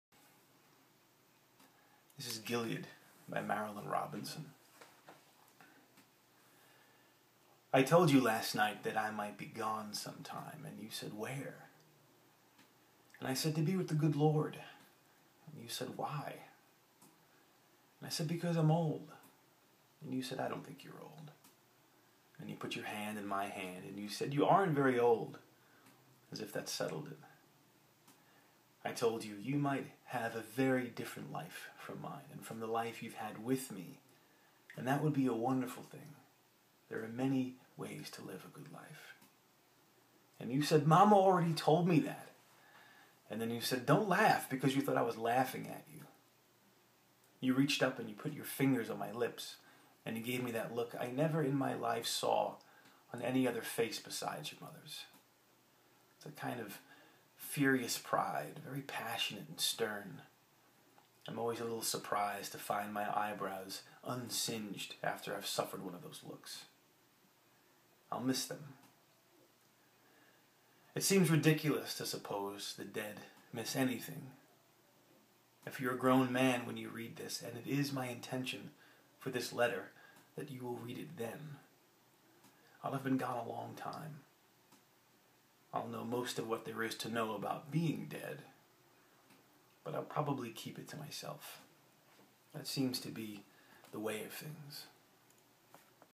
Scott Cheshire reading from Gilead by Marilynne Robinson
This video is for 90Outloud where those of us who believe in books try to convince the rest of us in 90 seconds or less.

Scott Cheshire reading Marilynne Robinson's Gilead available and other major bookstores.